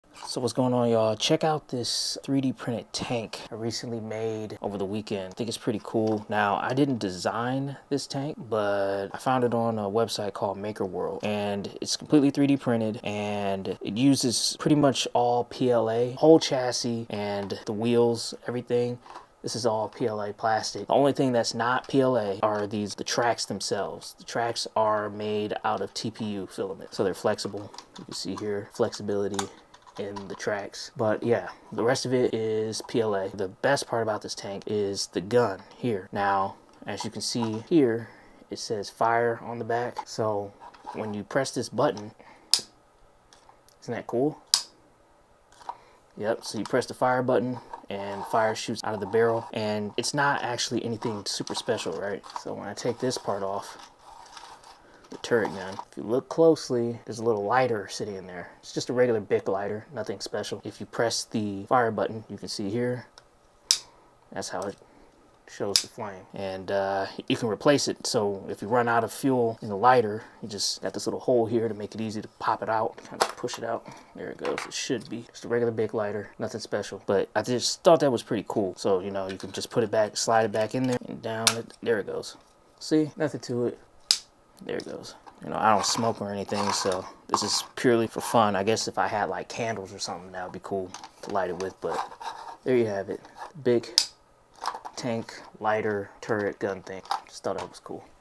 3D Printed Tank
Check out this tank I recently 3D printed. The gun barrel has an interesting feature to it. It took about 7+ hours to finish from beginning to end